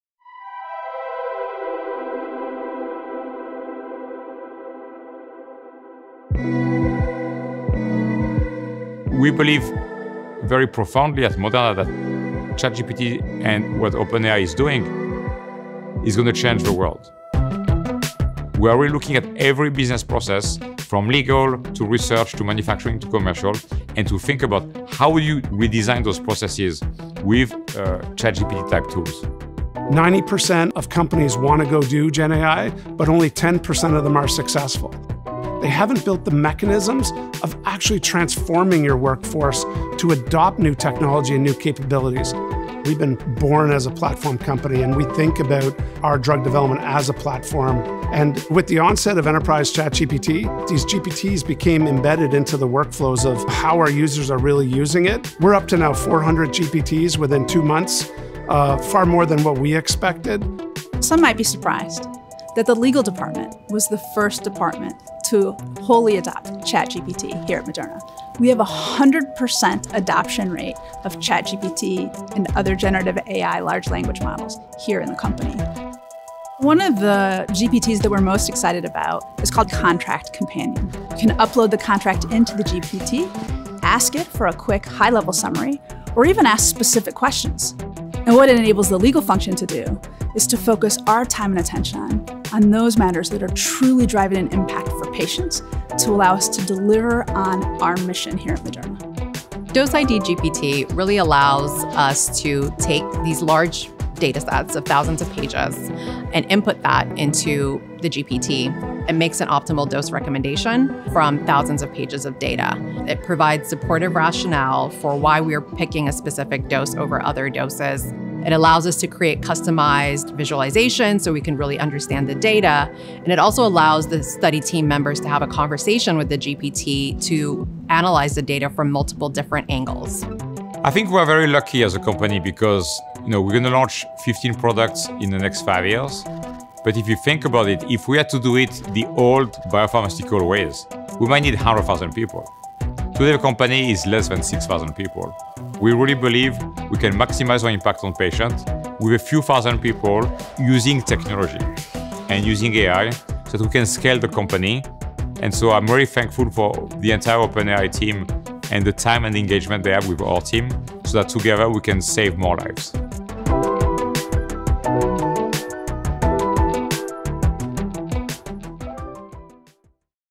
Moderna x OpenAI
We’re excited to announce our ongoing collaboration with @OpenAI to co-innovate with a shared vision of AI’s immense potential in the future of business and healthcare. AI and collaborations like this will be a key component to our ability to scale and maximize our impact on patients.

Watch the video below and read our blog to learn more about our work with OpenAI